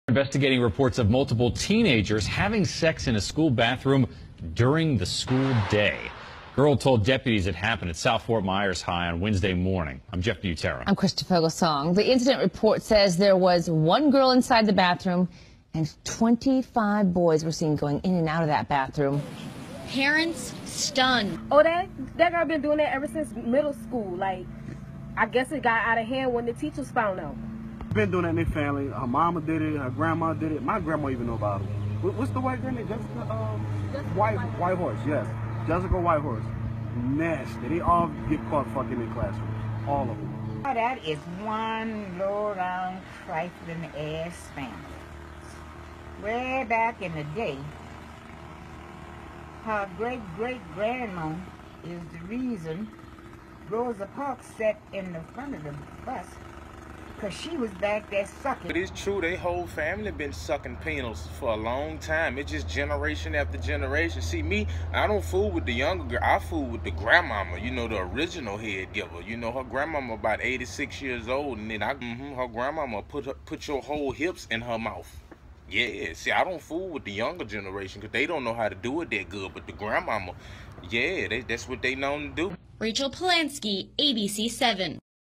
Gucci Robinson interviewed by the news
Teenagers having sex in a school bathroom